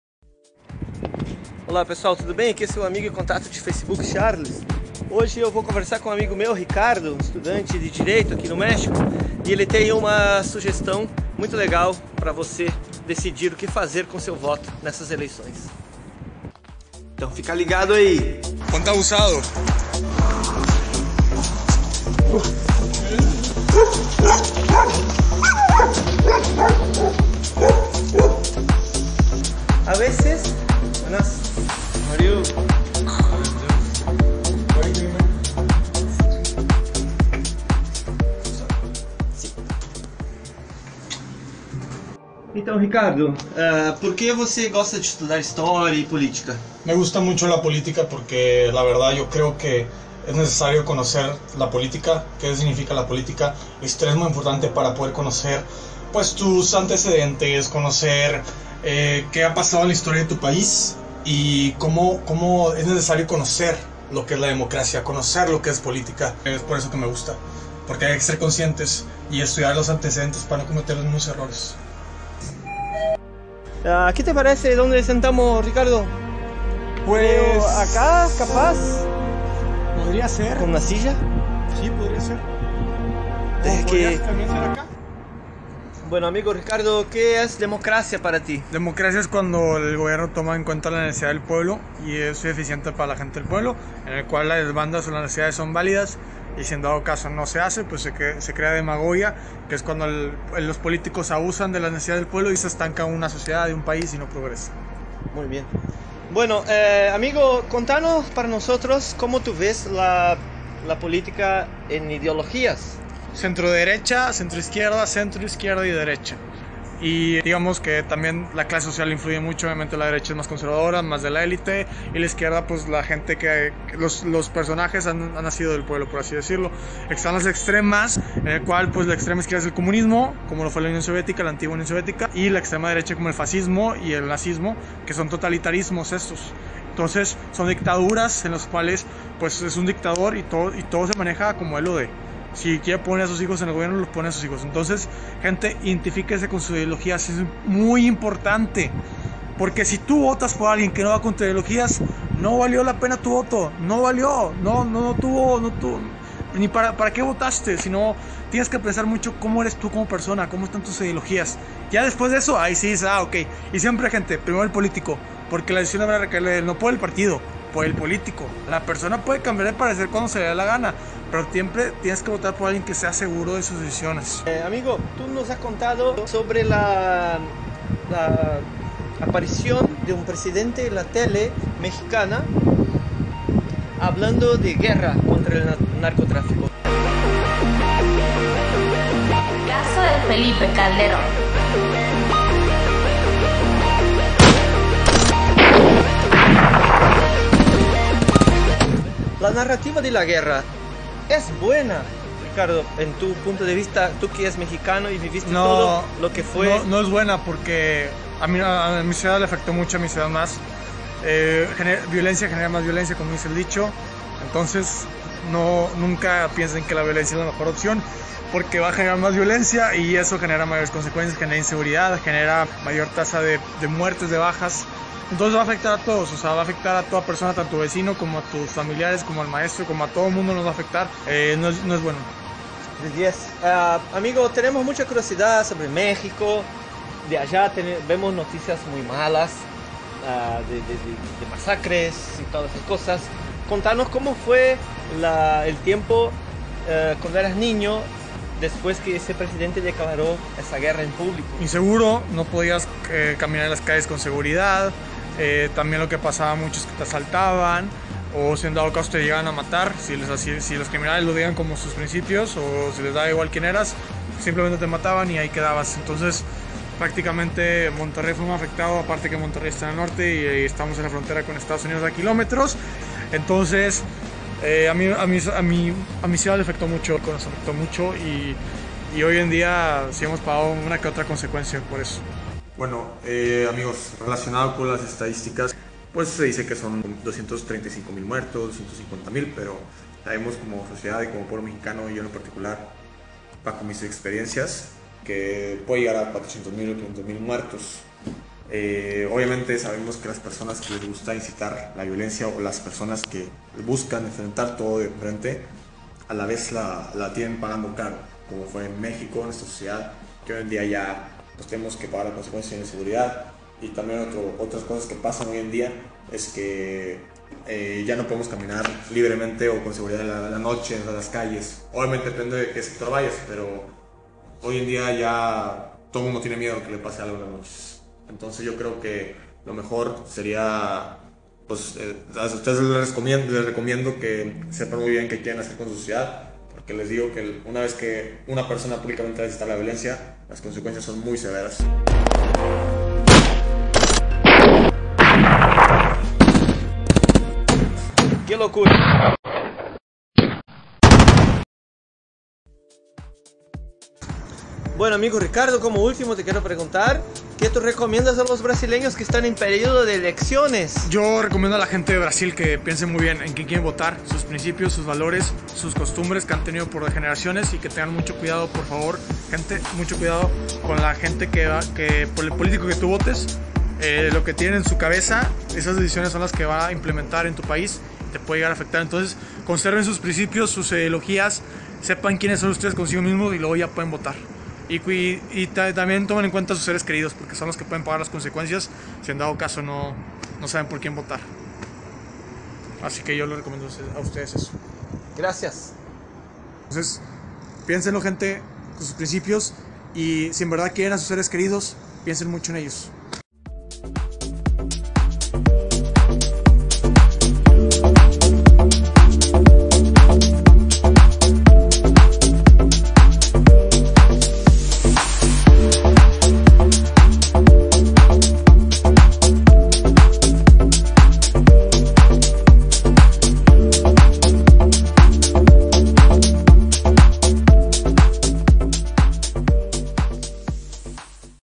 O argumento da violência arruina a sociedade. O exemplo mexicano de Calderón
Gravei este vídeo para tomar o RELATO DE UM MEXICANO, estudante de Direito, que viveu alguns efeitos da guerra aberta do governo do México contra o narcotráfico.
O problema é antigo, mas ficou pior por causa da postura belicista (vía argumento da violência) de um certo presidente mexicano, que terminou arruinando uma ferida que até hoje doi na sociedade, sem, entretanto, resolver o problema. Pelo contrário...
Muitos mexicanos morreram gratuitamente... Com vocês, um pouco de histórias de México para inspirar os brasileiros nessas Eleições de 2018.